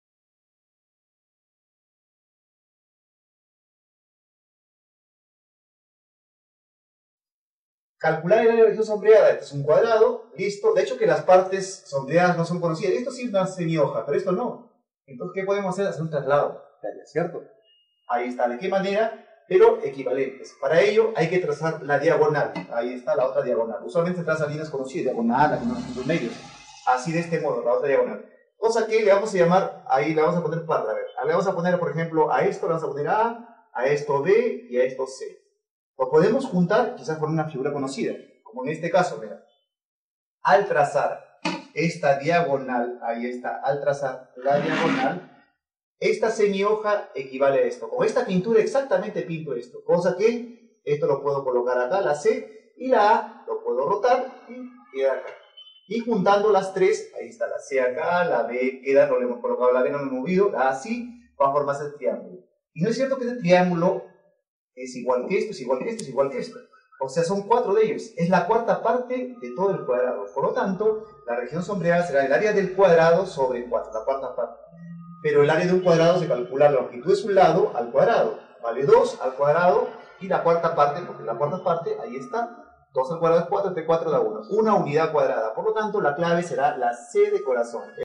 Examen Admisión a la Universidad Villareal Áreas de Regiones Sombreadas Solucionario UNFV
Áreas de Regiones Sombreadas  - Razonamiento geométrico Problema Resuelto de Geometría - matemáticas - Razonamiento Matemático , números y operaciones .

Prueba de  ingreso a la universidad nacional federico villareal - soluciones de Matemáticas .

Examen general Ordinario desarrollado  - todas las áreas -

El profesor Jorge Ayala  resuelve  una pregunta de examen de ingreso a la universidad de nivel preuniversitario en la Academia Rubiños .

clase  preuniversitaria y de secundaria expuesto en la academia de pre universidad rubiños .